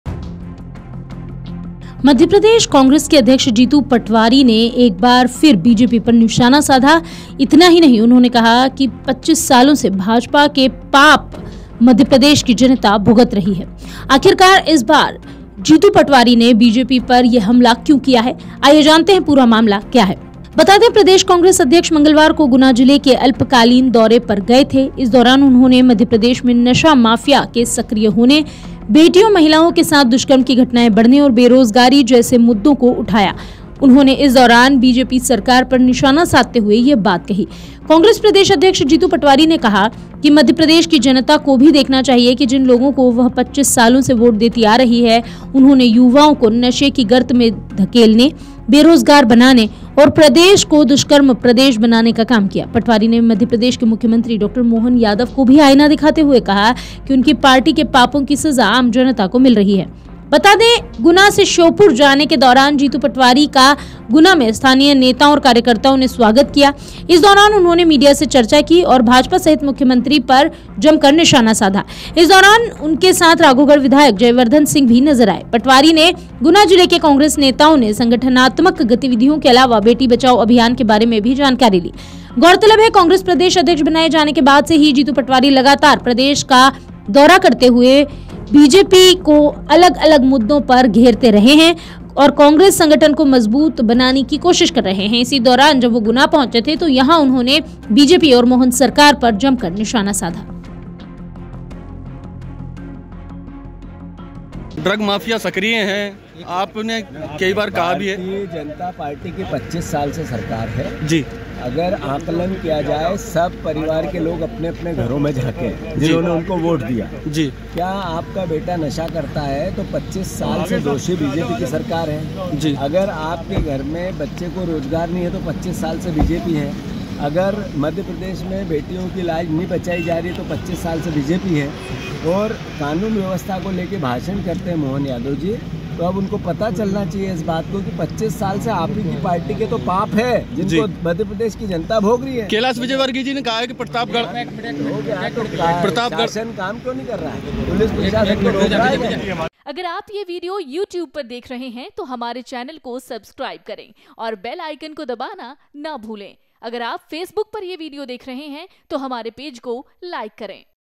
Guna पहुंचे PCC Chief Jitu Patwari, BJP और CM Mohan Yadav पर जमकर बरसे| Jaivardhan Singh |Amar Ujala
मध्यप्रदेश कांग्रेस के अध्यक्ष जीतू पटवारी ने कहा है कि प्रदेश में 25 सालों से शासन कर रही भाजपा सरकार के पाप आम जनता को भोगने पड़ रहे हैं। आपको बता दें प्रदेश कांग्रेस अध्यक्ष मंगलवार को गुना जिले के अल्पकालीन दौरे पर आए थे। इस दौरान उन्होंने मध्यप्रदेश में नशा माफिया सक्रिय होने, बेटियों-महिलाओं के साथ दुष्कर्म की घटनाएं बढऩे और बेरोजगारी जैसी मुद्दों को लेकर भाजपा पर निशाना साधते हुए यह बात कही है।

Madhya Pradesh Congress President Jeetu Patwari has said that the common people are suffering the sins of the BJP government which has been ruling the state for 25 years. Let us tell you that the state Congress President came on a short visit to Guna district on Tuesday. During this, he said this while targeting the BJP on issues like drug mafia being active in Madhya Pradesh, increasing incidents of rape with daughters and women and unemployment.